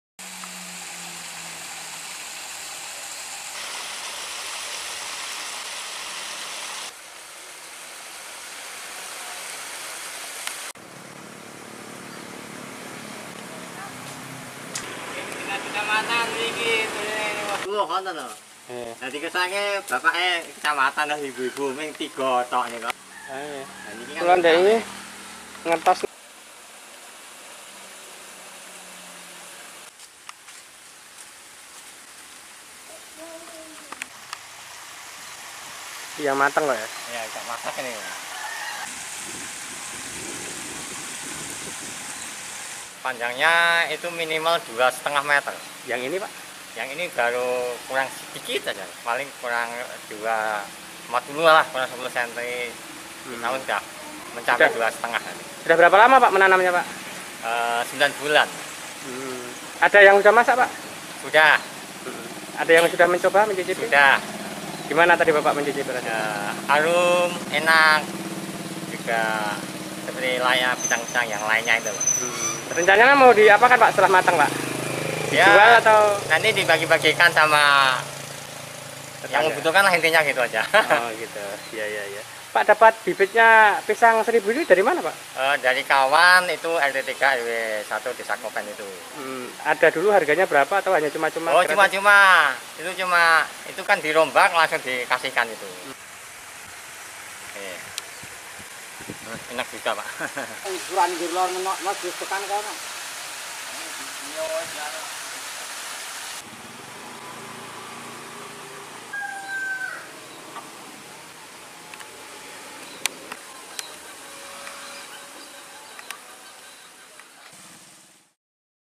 Viral! Berbuah Ribuan, Pohon Pisang Ini Sukses Curi Perhatian Warga
LIPUTAN6.COM - Viral! Berbuah Ribuan, Pohon Pisang Ini Sukses Curi Perhatian Warga

Pohon pisang milik warga Dukuh Bontitan Desa Kopen, Kecamatan Teras, Boyolali, Jawa Tengah ini memiliki jumlah buah pisang yang mencapai ribuan. Banyaknya buah membuat pohon pisang tersebut disangga dengan batang bambu agar tidak roboh.